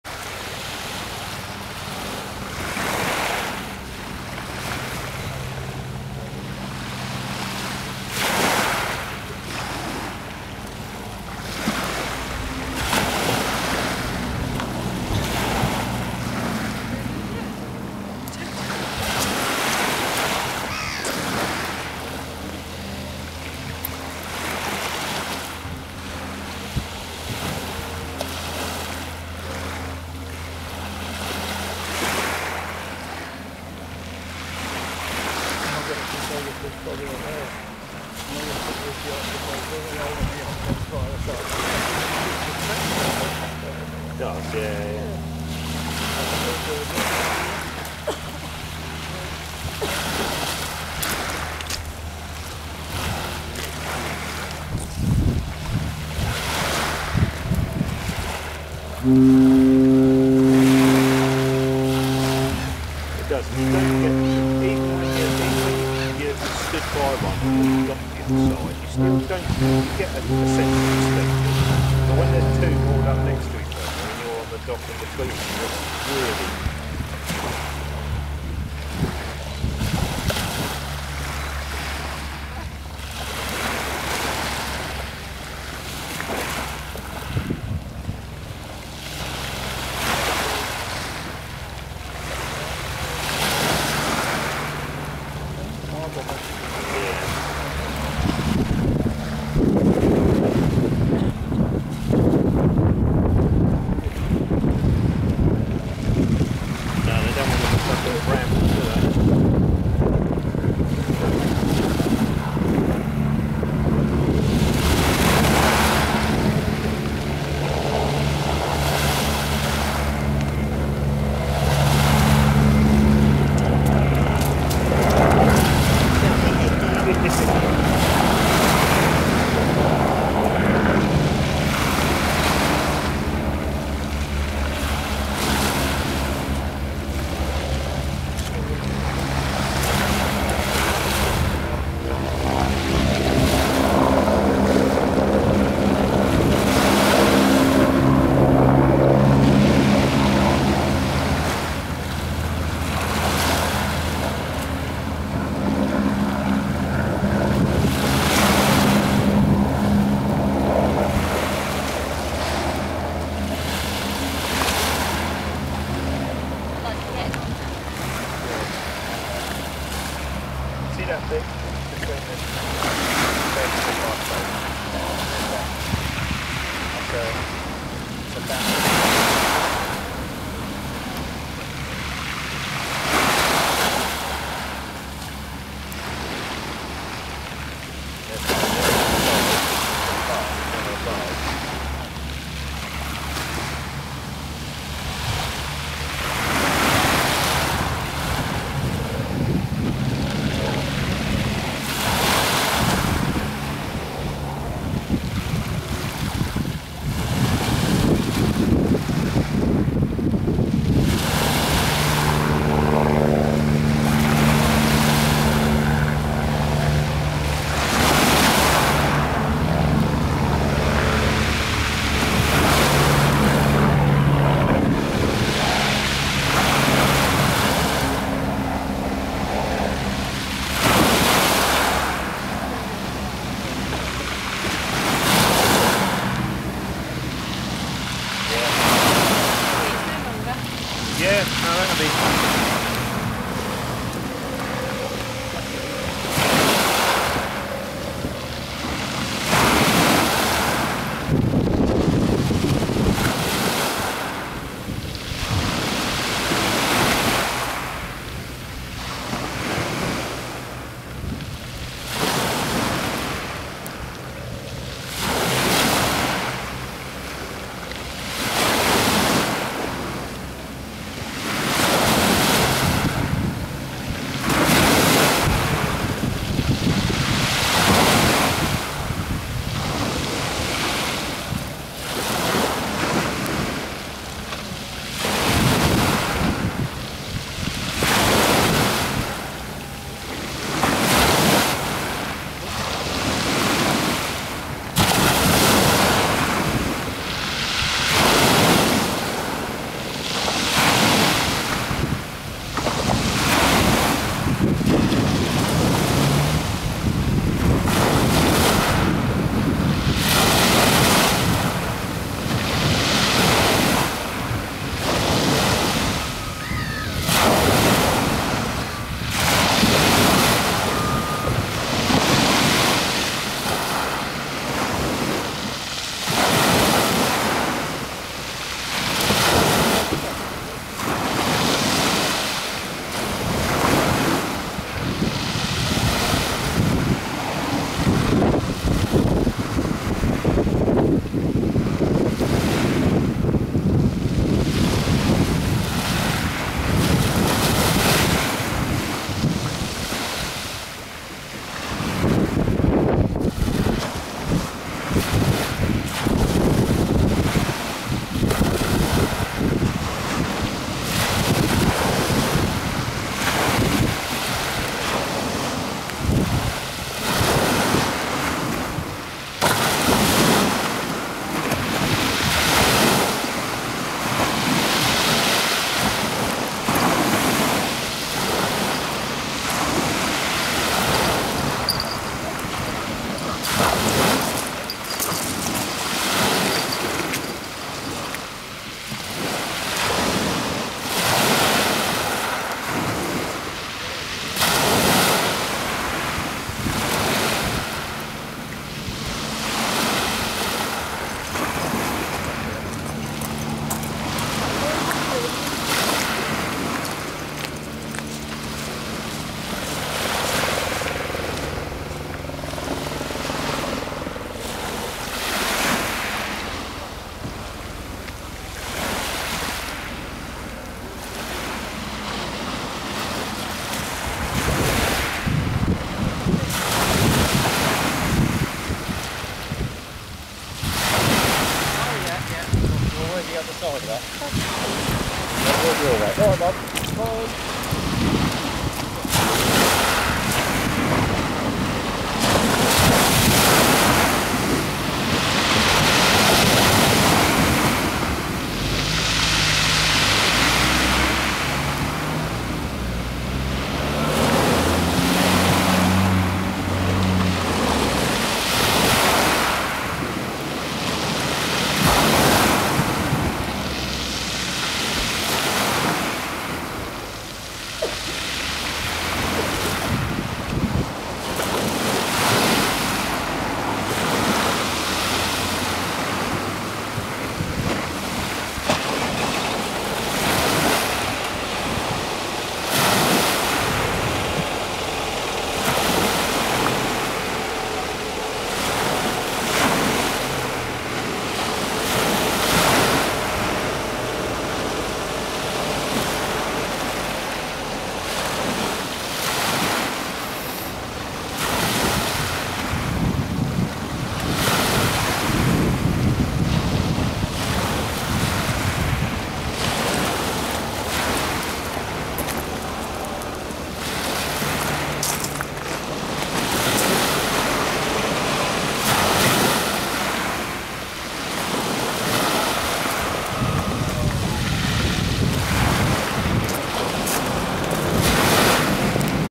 P&O BRITANNIA on her first sailing into Southampton turning off the Brambles Bank COWES
First ever viewing CLOSE UP of P&O BRITANNIA new cruise ship 6th March 2015...